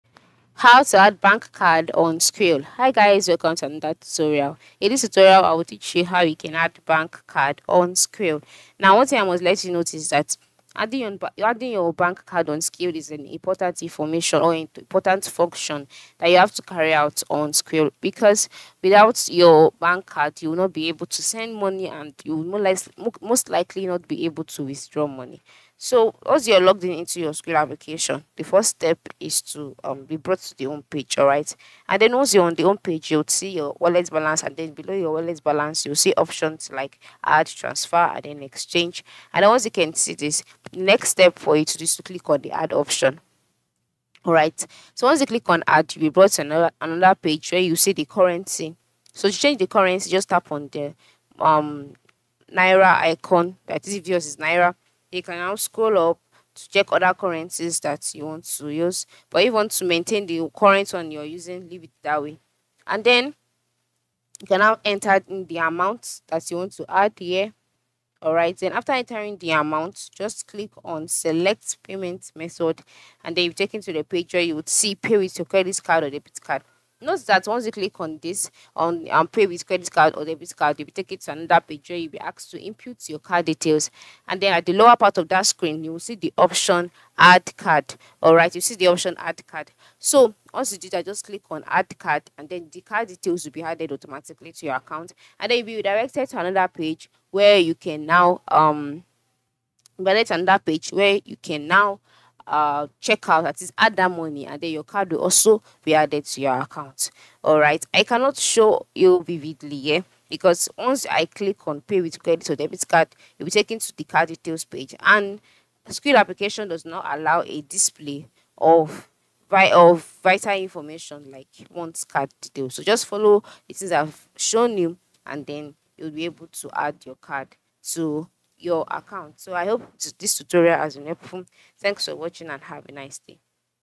How To Add Bank Card On Skrill (Quick Tutorial)​
Today we talk about How To Add Bank Card On Skrill, so stay until the end of the video to see the full explanation.